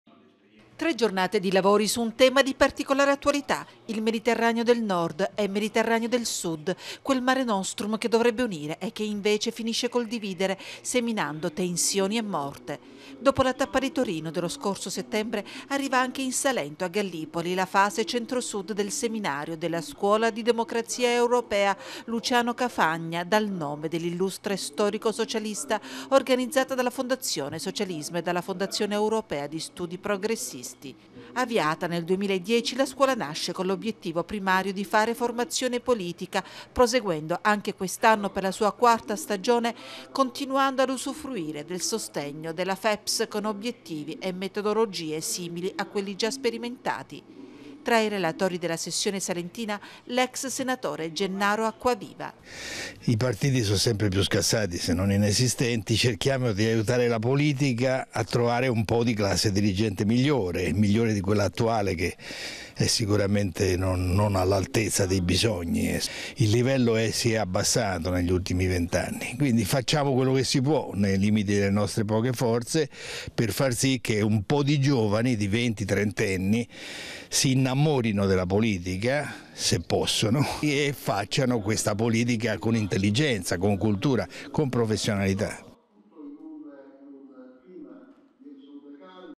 GALLIPOLI CONVEGNO SCUOLA DI DEMOCRAZIA
A GALLIPOLI ARRIVANO I SEMINARI DELLA SCUOLA DI DEMOCRAZIA EUROPEA. OBIETTIVO_ AIUTARE LA CLASSE POLITICA A FORMARE LA NUOVA CLASSE DIRIGENTE.